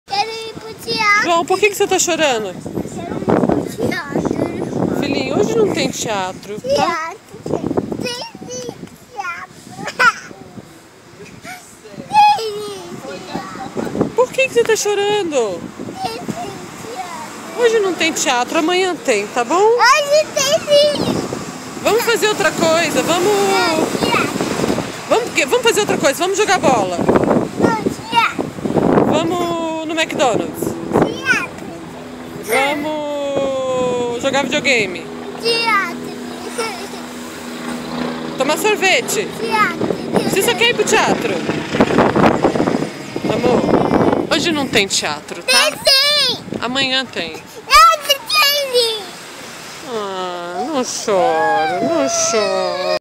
TEATRO!!!
Chocolate?
Video Game?

João Antônio quer muito, mas muito ir ao teatro.